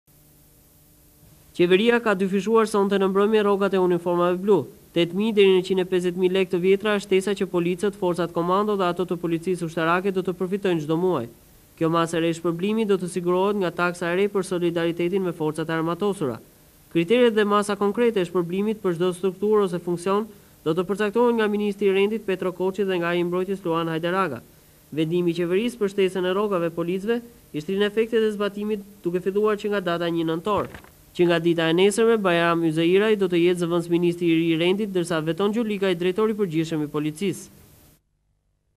Lajme Arkiva - Qeveria rrit pagat e punonjesve te policise (07 Nentor 1998)
Arkiva - Tv Klan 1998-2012; Pamje kryeministri Pandeli Majko mbledhje qeverie, pamje tanke ushtarake.

Qeveria në vendimin e saj të fundit deklaroi se janë rritur rrogat e uniformave blu. Shtesat që do të kenë policët, forcat komando dhe ato të policisë ushtarake , do të jetë nga 8 mijë deri në 150 mijë lekë të vjetra. Kjo masë e re shpërblimi do të sigurohet nga taksa e re për solidaritetin e forcave të armatosura. Kriteret dhe masat konkrete e shpërblimit dhe të strukturës në funksion, do të përcaktohet nga ministri i Rendit Petro Koçi dhe ai i Mbrojtjes Luan Hajdaraga. Nga dita e nesërme Bajram Yzeiraj do të jetë zëvëndësministri i ri i vendit ndërsa Veton Gjolikaj Drejtor i Përgjithshëm i Policisë.